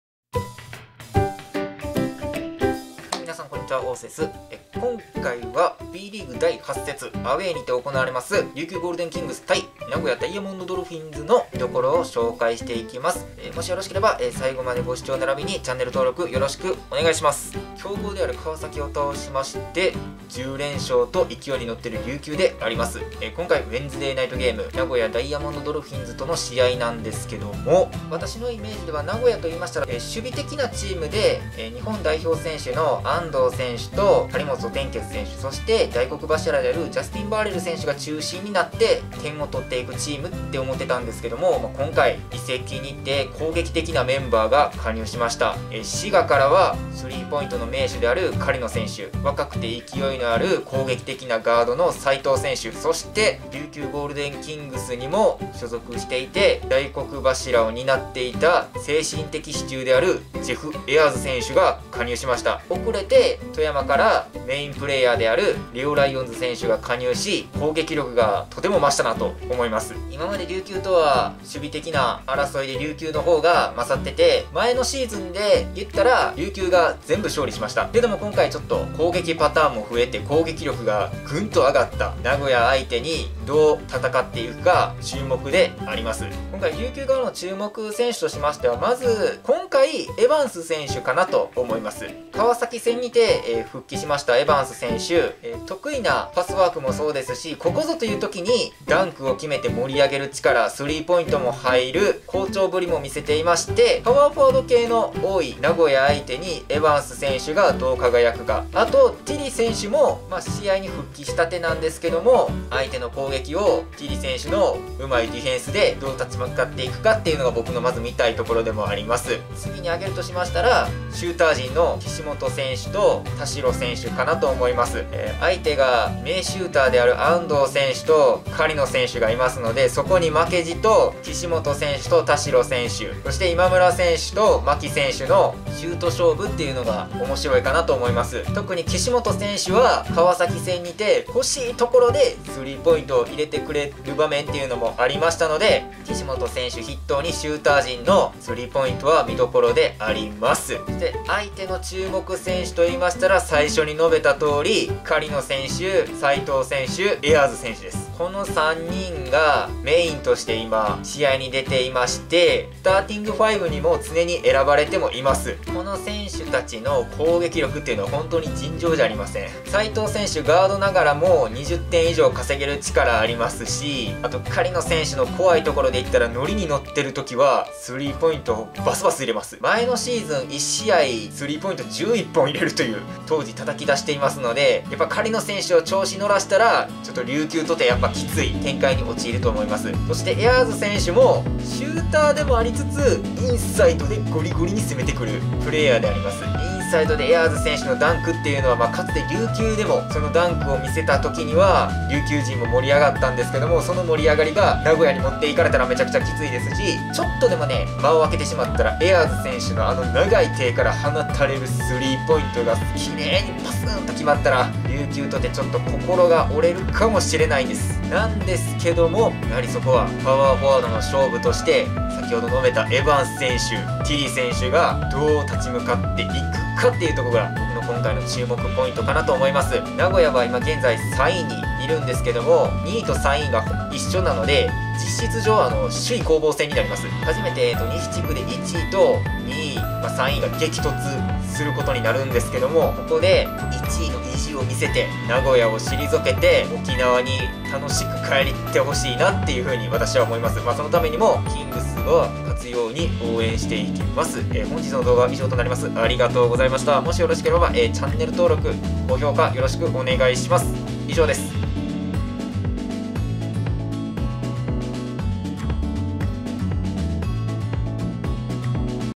琉球ゴールデンキングス Bリーグ第8節 見どころ VS名古屋ダイヤモンドドルフィンズ
首位の座を狙っている名古屋ダイヤモンドドルフィンズ戦の見どころ紹介です。
攻守ともにパワーアップした名古屋の首位攻防戦の激戦必須です。

※齋藤選手の齋がフォントの都合上変換ができなかったので動画上簡単の齋藤表記になっています事ご理解願います。